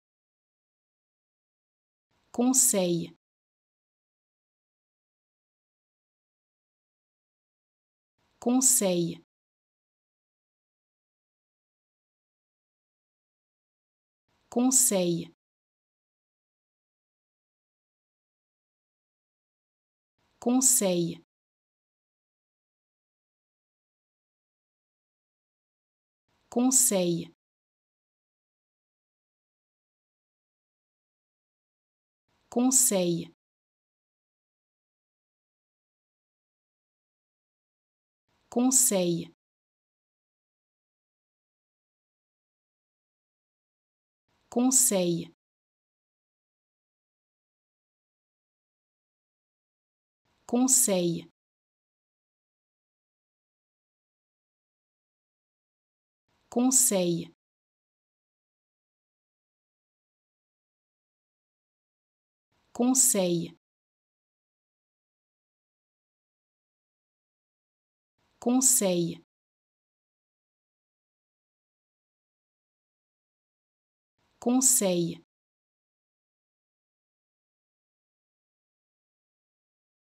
How to Pronounce ''Conseil'' Correctly in French
Learn how to say and properly pronounce ''Conseil'' in French with this free pronunciation tutorial. Apprenez à prononcer des mots en français grâce à nos tutoriels.